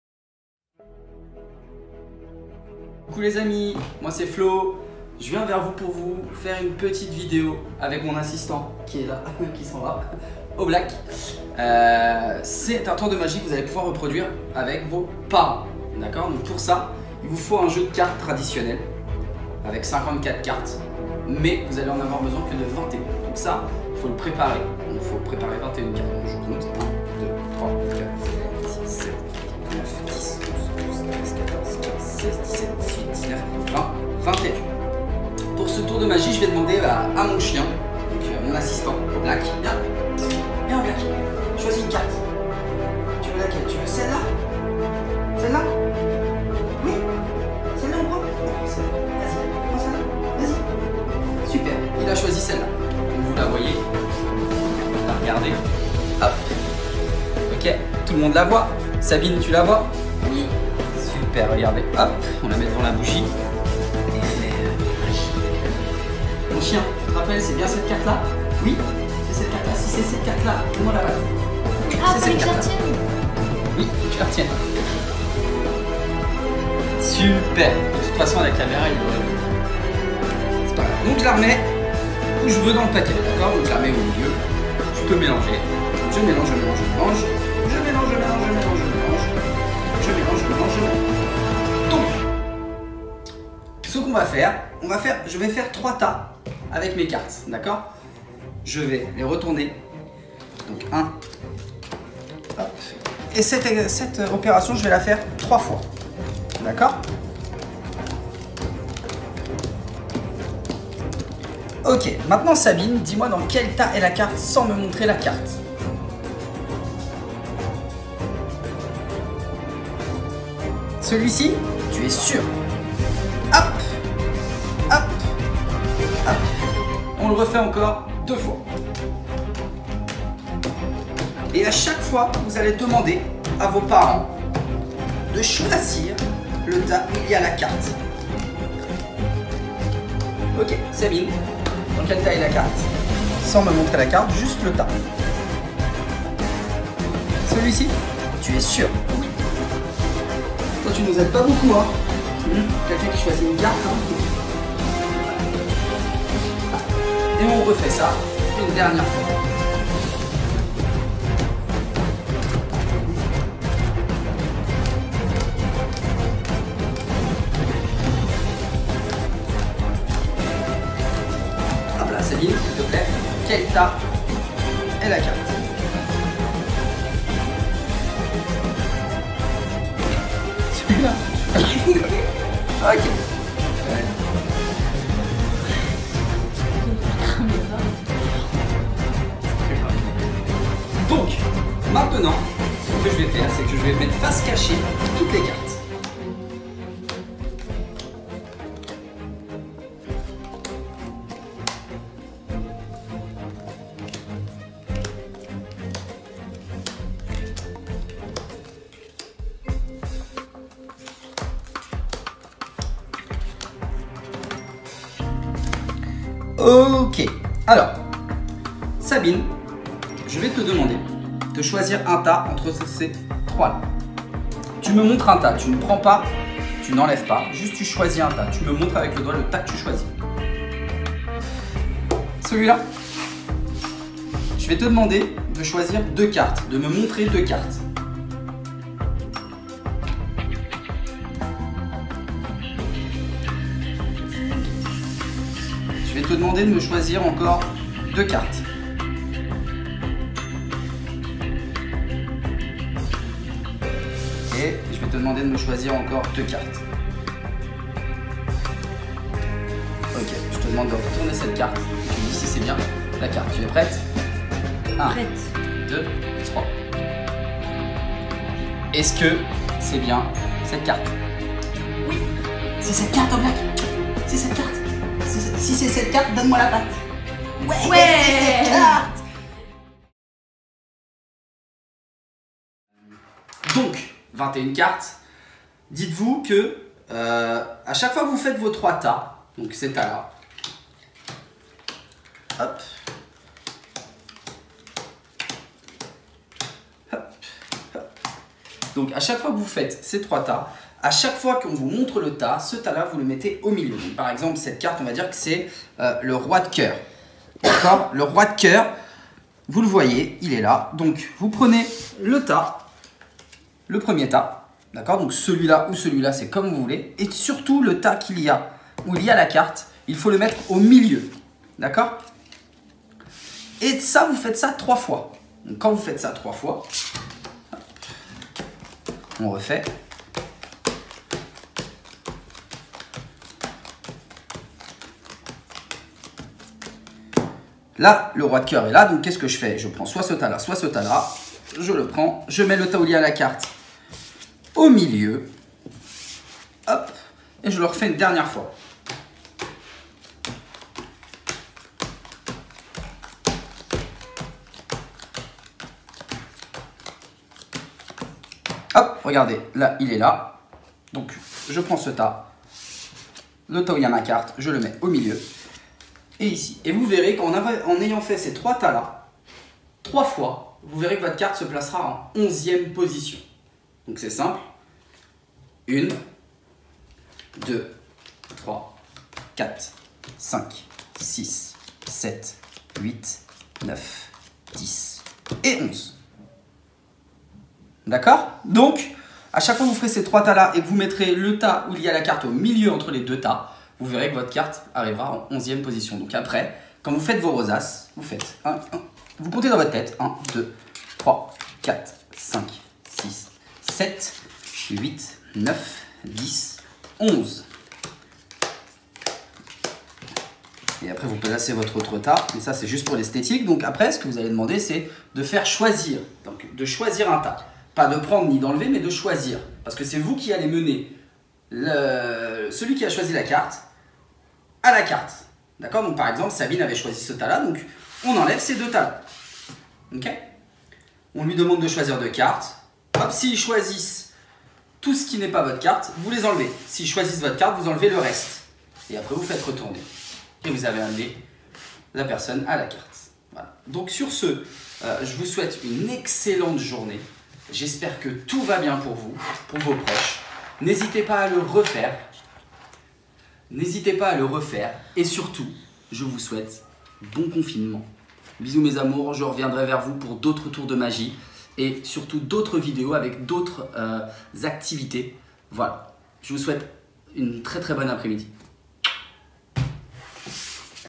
[Jean Rostand /Les Champagnes] ♠♦Tour de magie - La carte insaisissable - Episode 1♠♦
😊Florient  ,  animateur à l'école Jean Rostand /Les Champagnes, vous propose aujourd'hui un tour de magie - La carte insaisissable - Episode 1 ♠♦ .